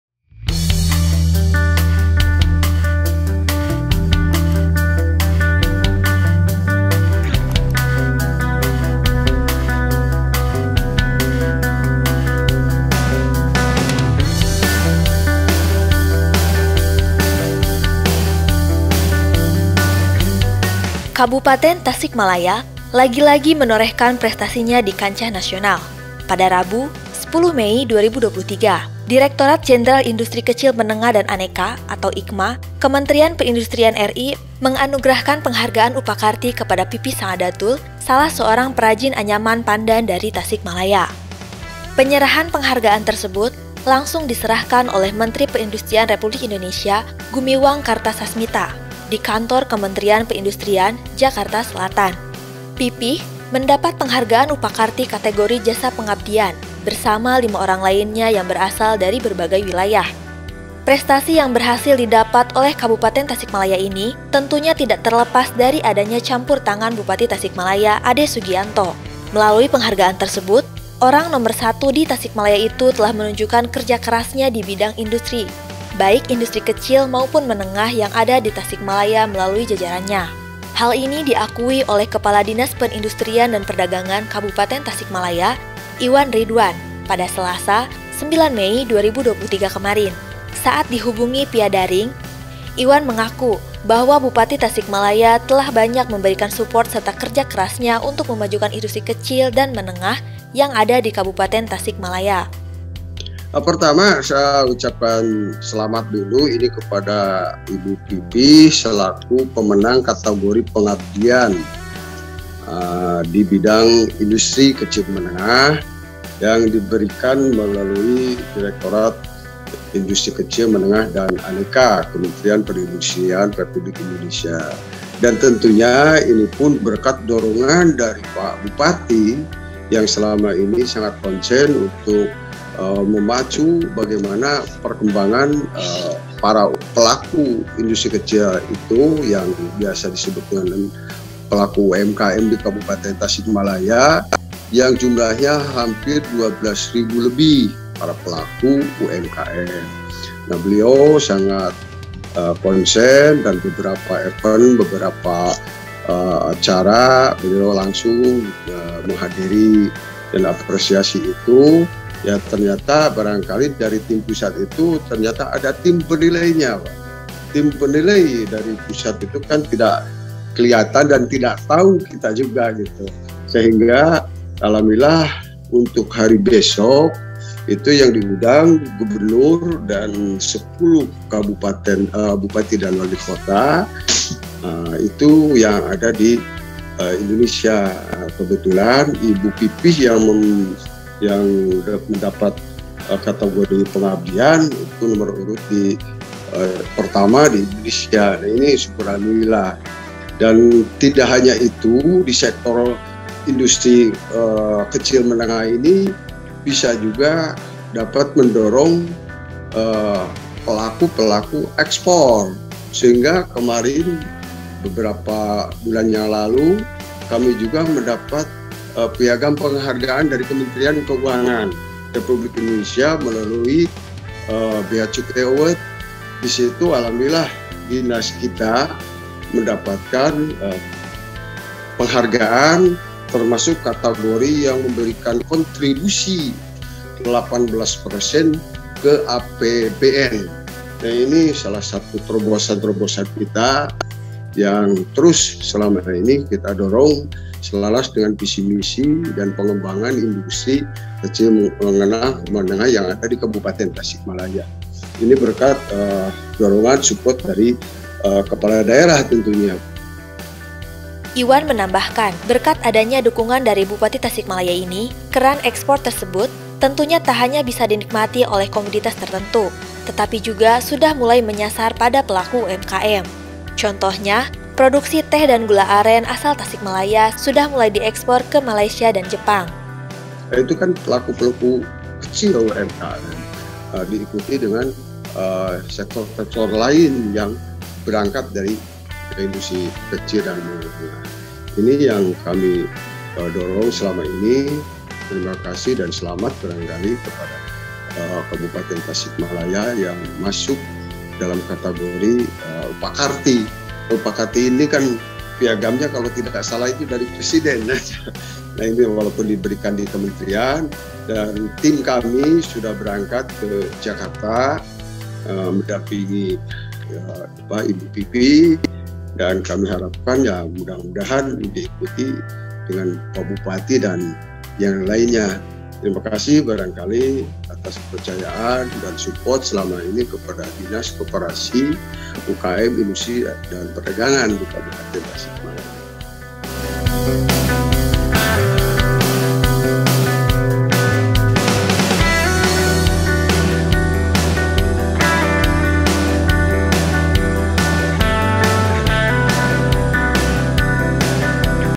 Ade Sugianto Diganjar Upakarti; Penghargaan Paling Prestisius dalam Bidang UMKM
Pada Rabu, 10 Mei 2023, Direktorat Jenderal Industri Kecil Menengah dan Aneka (IKMA) Kementerian Perindustrian RI, menganugerahkan penghargaan Upakarti kepada Pipih Saadatul, salah seorang perajin anyaman pandan dari Tasikmalaya.